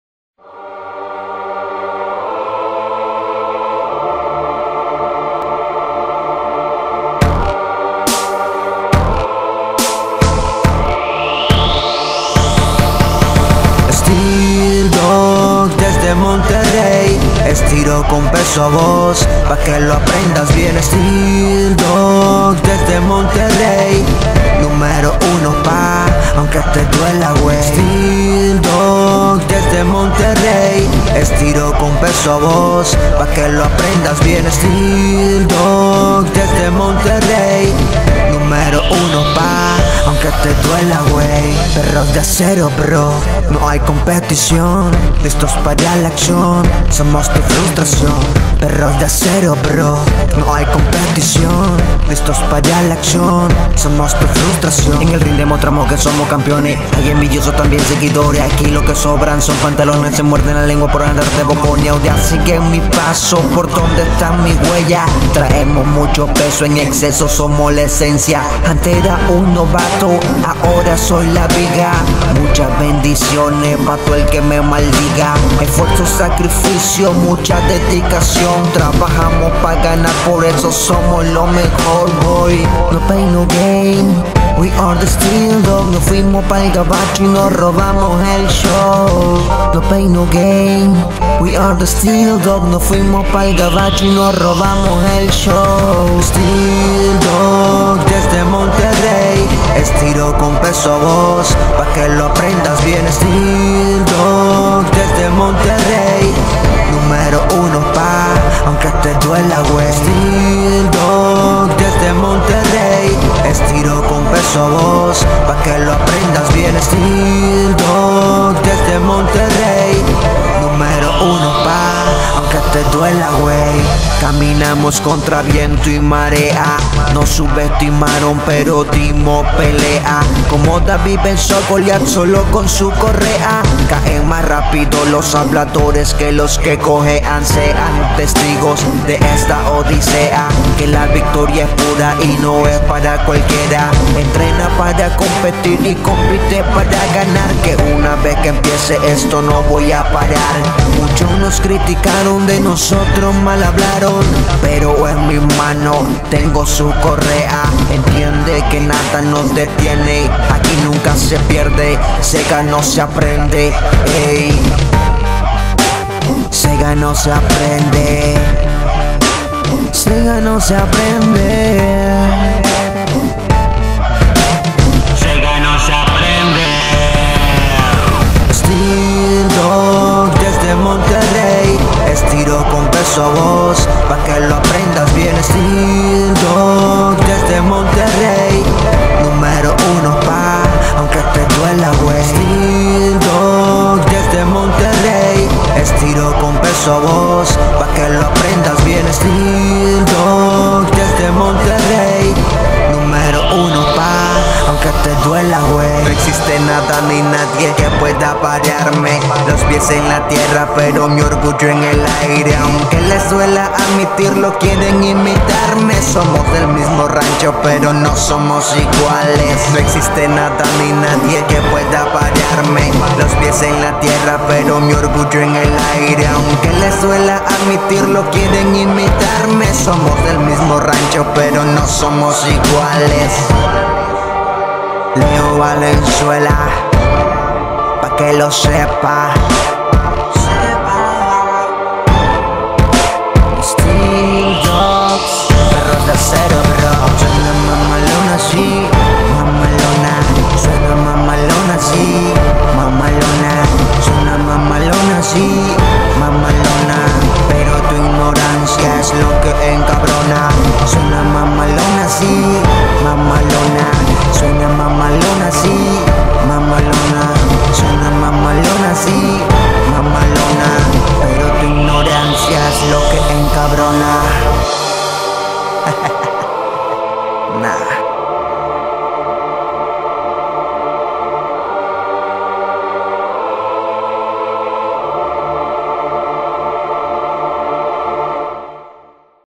Steel Dogs | Weight Pulling
Steel Dogs es un grupo creado para practicar el arrastre de peso con nuestros perros, abarcando todas las razas aptas para esto, enfocados en hacerlo a la voz de mando para que el perro lo desarrolle como un trabajo y no como una obsesión.